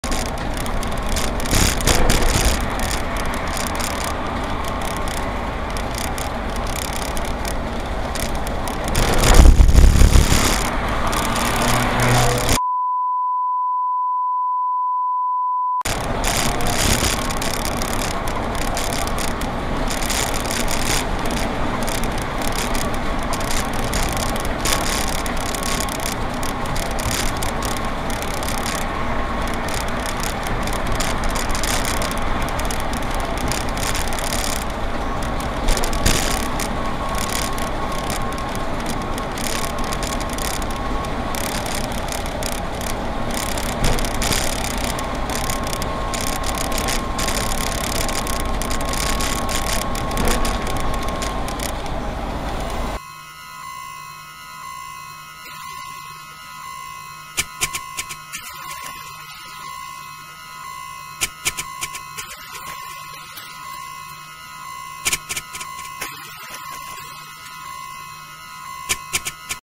Oszołom na buspasie
omija korek uprzednio ładując się pod autobus pełny ludzi, ulica Puławska kierunek Piaseczno.